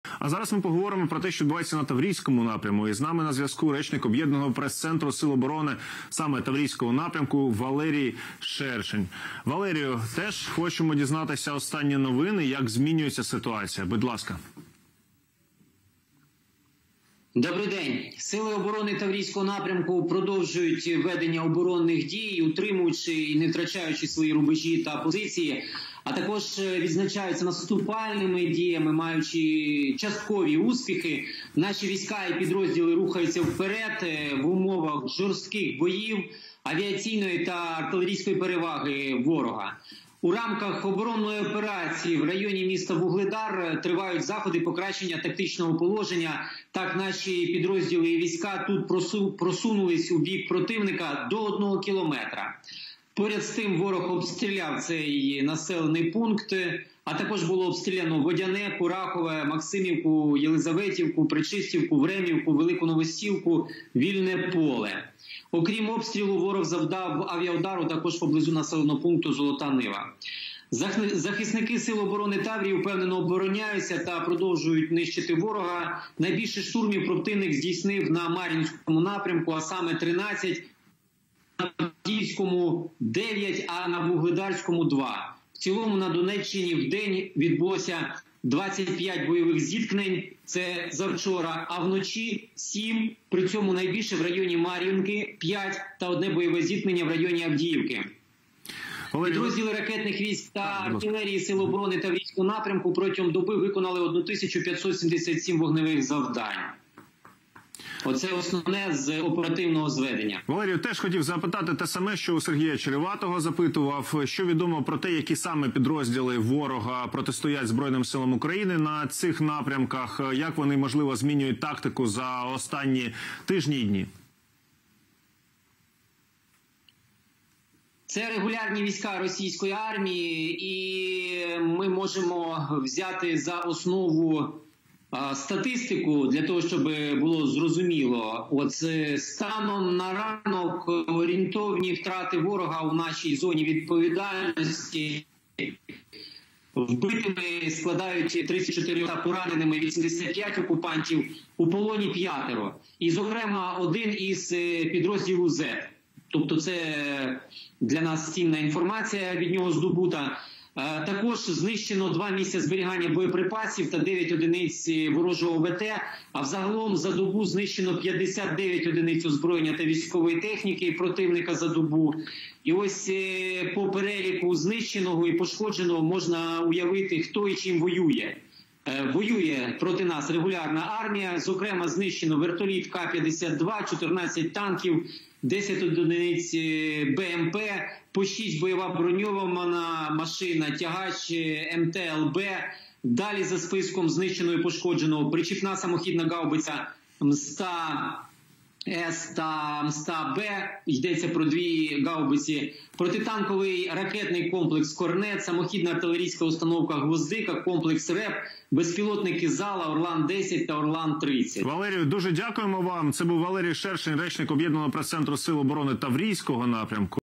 Українські військові просунулися у районі Вугледара на кілометр – Сили оборони
Під Вугледаром Сили оборони Таврійського напрямку просунулися вперед на відстань до 1 км. Про це в ефірі телемарафону «Єдині новини» повідомив речник об'єднаного пресцентру Сил оборони Таврійського напрямку Валерій Шершень.

«У рамках оборонної операції в районі міста Вугледар тривають заходи тактичного положення. Так, наші підрозділи і війська просунулись у бік противника до 1 км», – поінформував речник.

Водночас, за його словами, ворог обстріляв Вугледар, Водяне, Курахове, Максимівку, Єлизаветівку, Причистівку, Вемівку, Велику Новосілку, Вільне поле. Крім того, ворог завдав авіаудару поблизу н.п. Золота Нива. Найбільше штурмів ворог здійснив на Мар'їнському напрямку – 13, на Авдіївському – 9, на Вугледарському – 2.